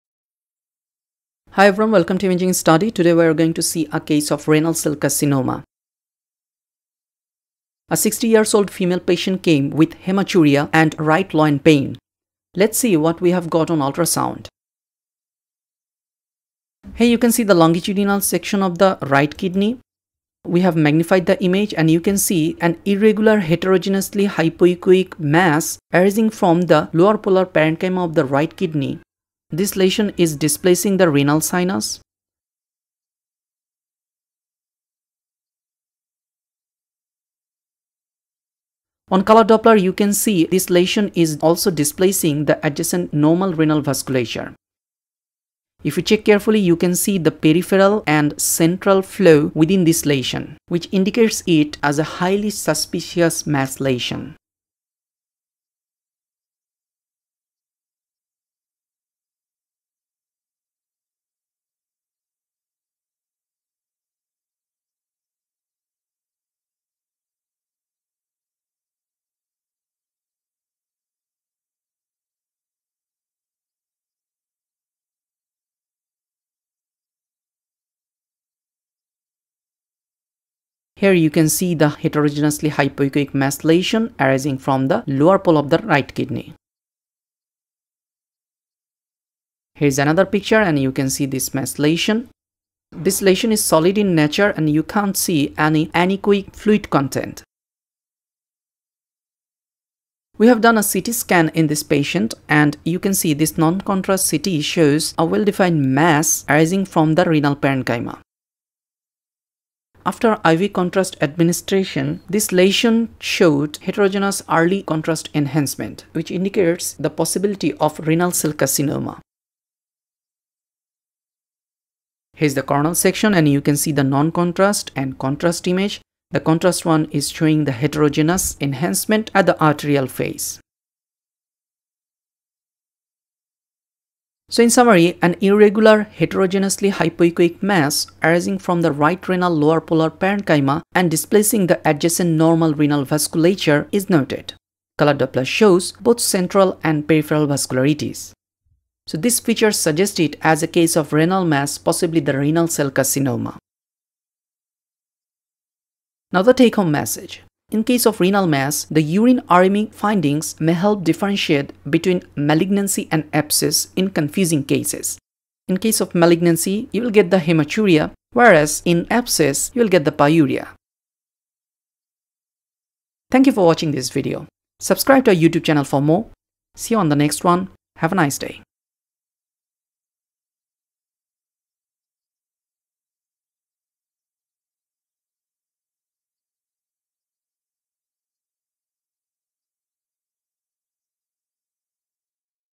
Renal Cell Carcinoma - RCC || Ultrasound || CT || Case 197
Clinical Features:
A 60 years old female patient came with
- Hematuria.
- Right loin pain.

Ultrasound Features:
- An irregular heterogeneously hypoechoic mass arising from the right renal lower polar parenchyma & displacing the adjacent normal renal vasculature.
- Color Doppler shows both central & peripheral vascularity.

Remember:
In case of renal mass, the urine RME findings may help differentiate between malignancy & abscess.